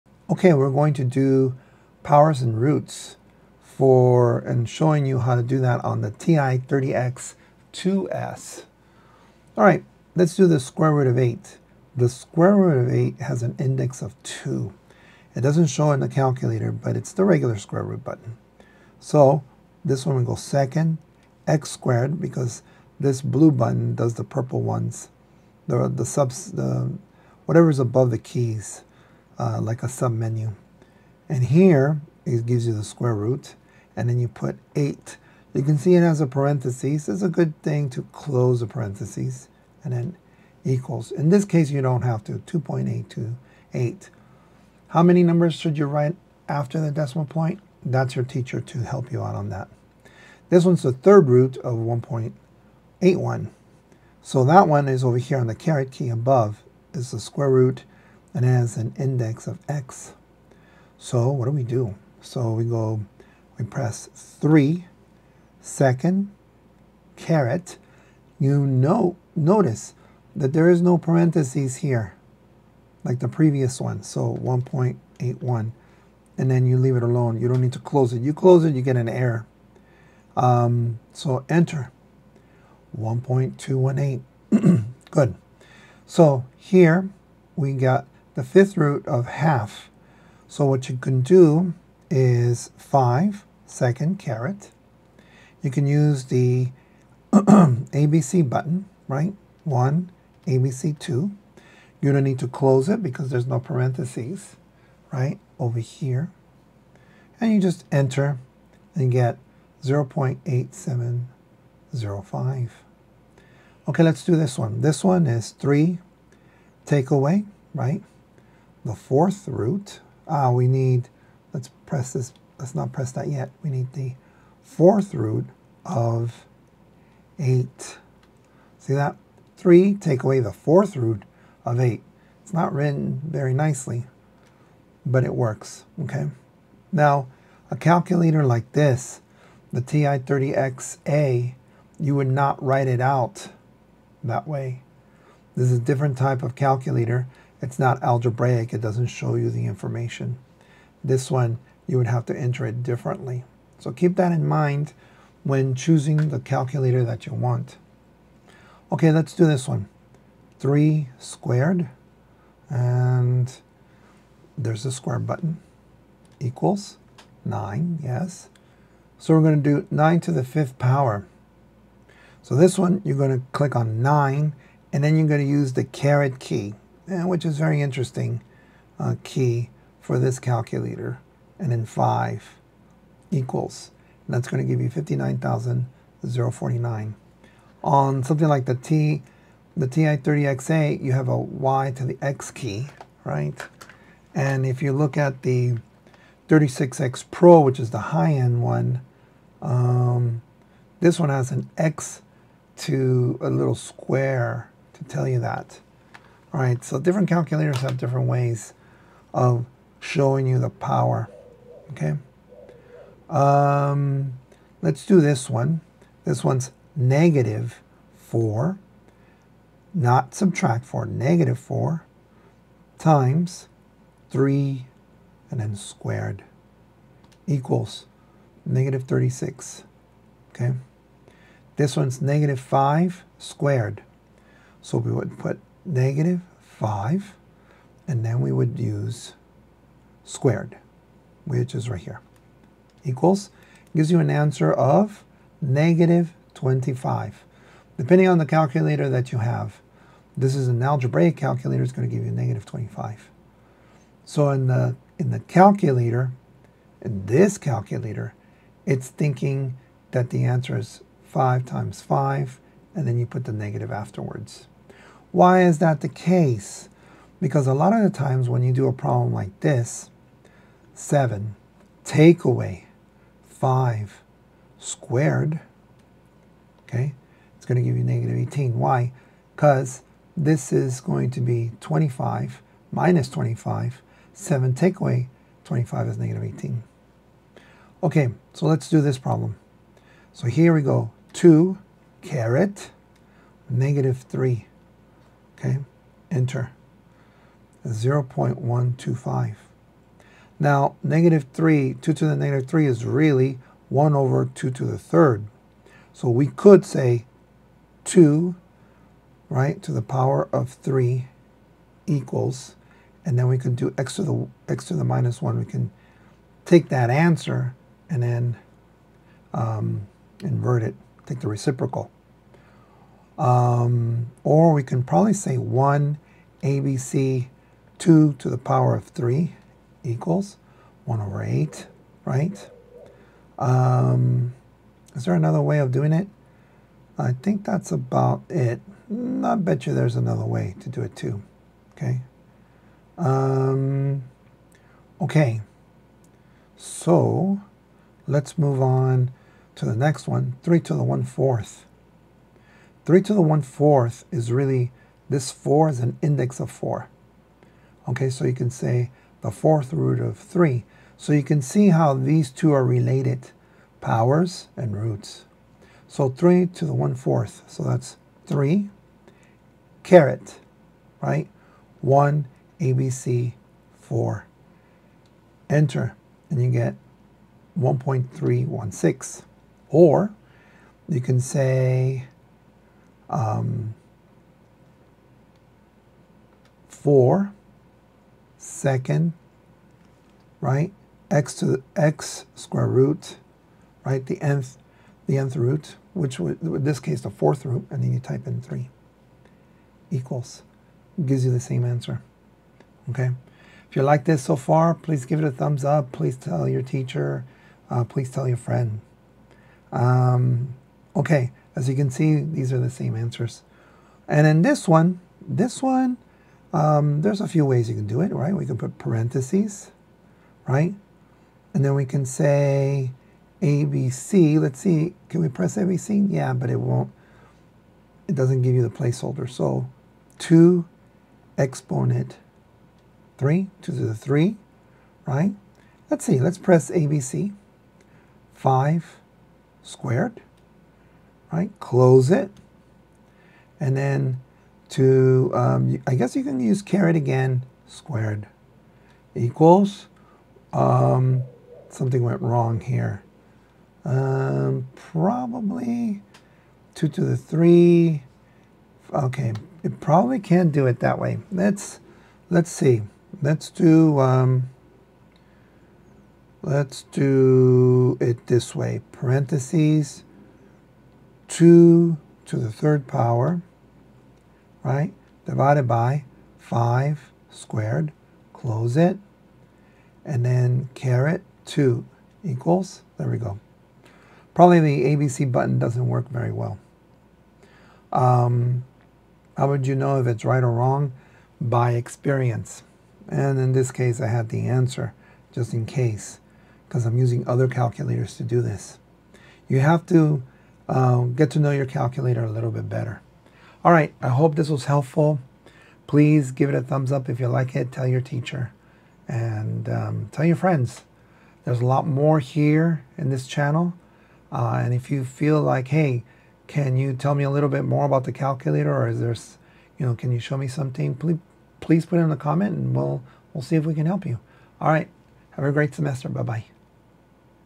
TI-30XIIS Exponents Radicals and Roots
In this video, we dive into using a scientific calculator for solving roots and powers in math problems. Whether you're dealing with square roots, cube roots, or higher powers, I'll walk you through the step-by-step process to efficiently handle these calculations on your calculator. This tutorial is perfect for students, beginners, and anyone needing a quick refresher. By the end, you'll confidently solve any root or power problem with ease using your calculator. Let's make math easier together!